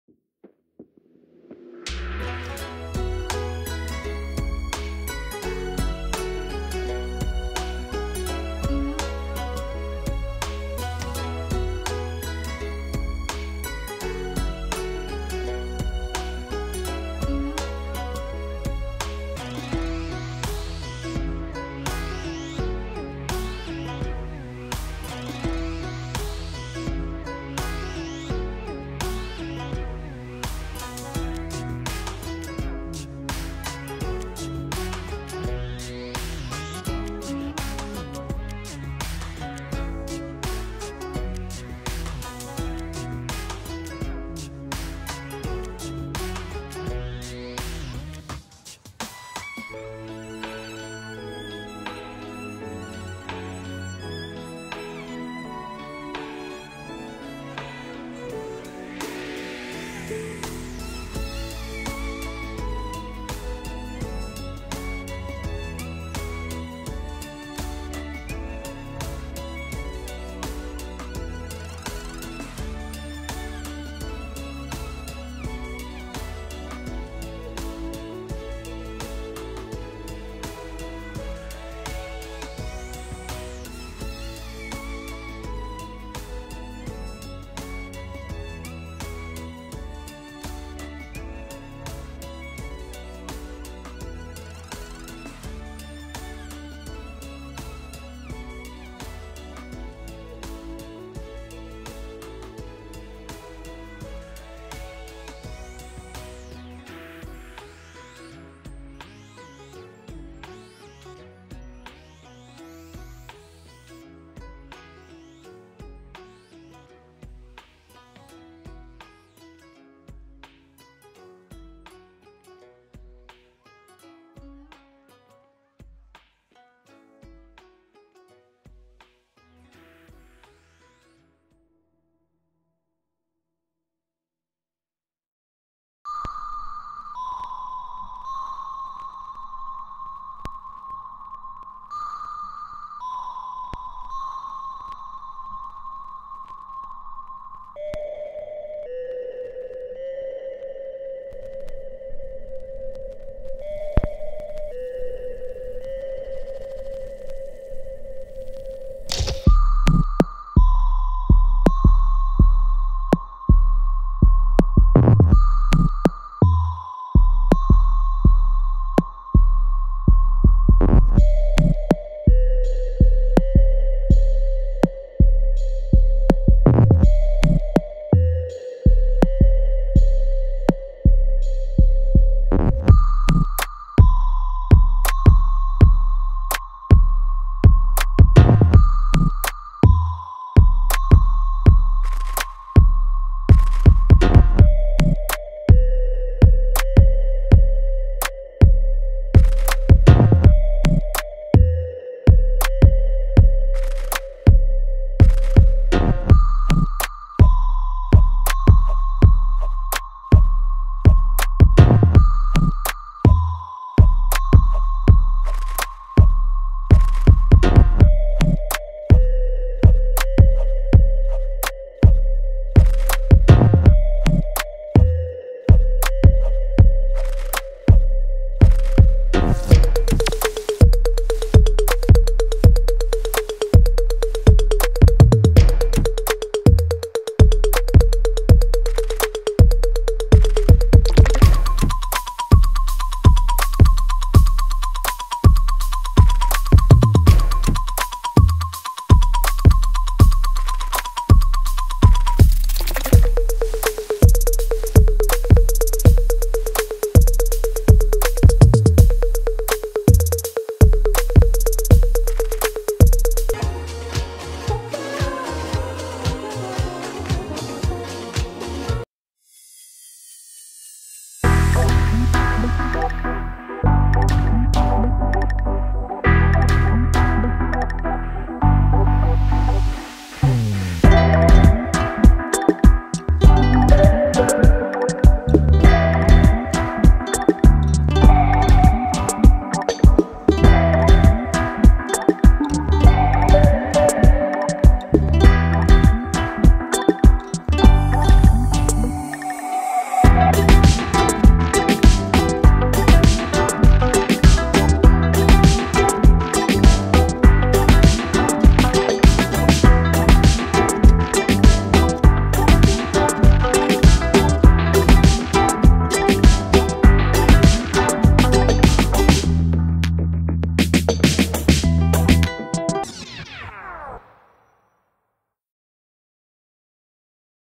Drawing NARUTO illustration making【procreate】
I drew my favorite NARUTO.
I will draw a lot and upload it.